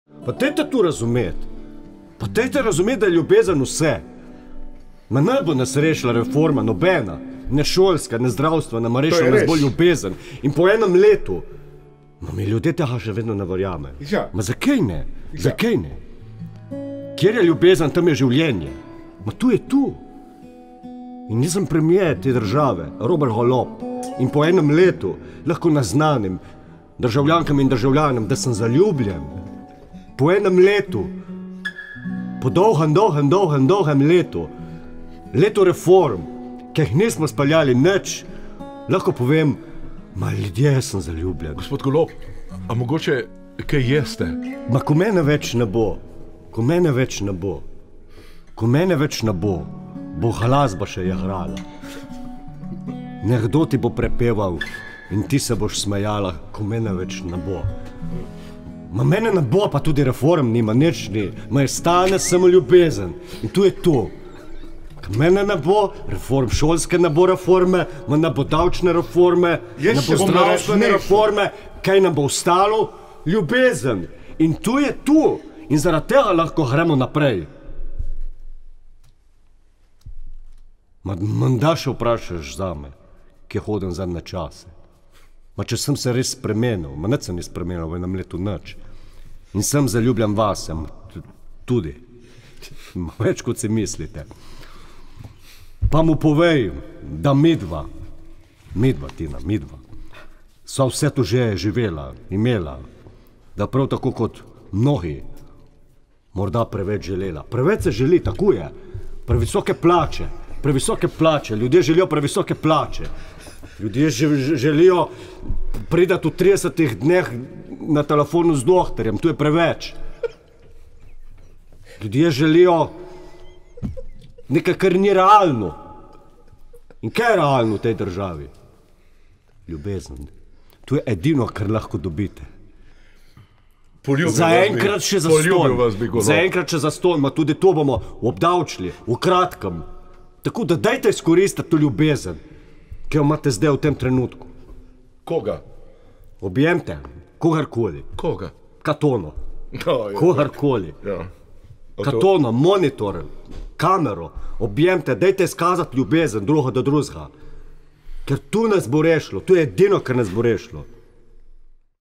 Golob o ljubezni ❤️
Ljubezen je edino, kar nam še preostane, pravi naš zaljubljeni premier - in recitira pesem "Ko mene več ne bo" ...